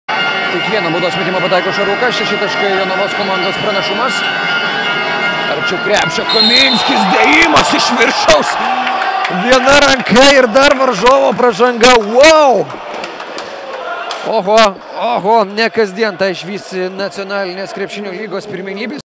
Ovidijus Kaminskis skrydį per varžovų galvas užbaigia dėjimu
"Telšių" komandos žaidėjas Ovidijus Kaminskis reidą arčiau Jonavos "Petrochemos-Dexteros" krepšio užbaigia galingu dėjimu.